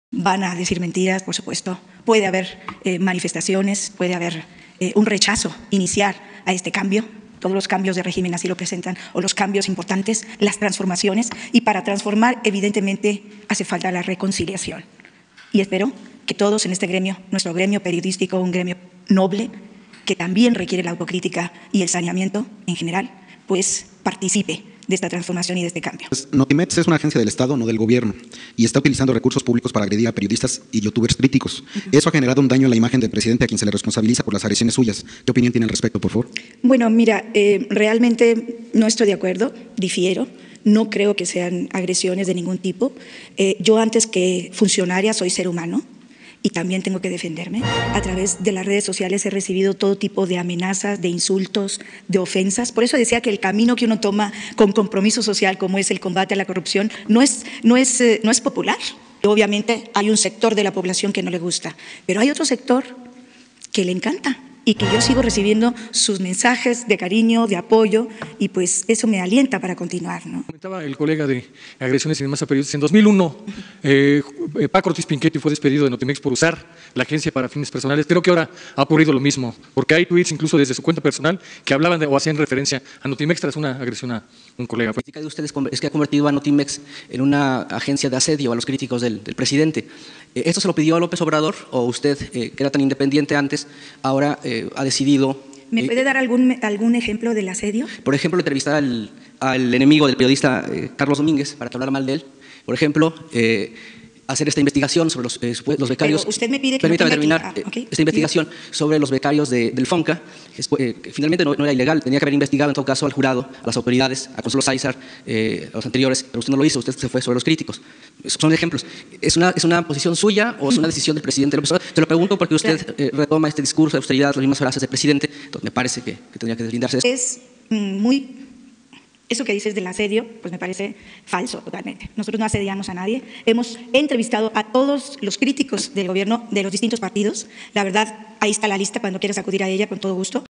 Sanjuana hace un llamado a la “reconciliación”
Sanjuana Martínez hace un llamado a la reconciliación del gremio negando que ella asedia y señala a otros periodistas usando recursos públicos de @Notimex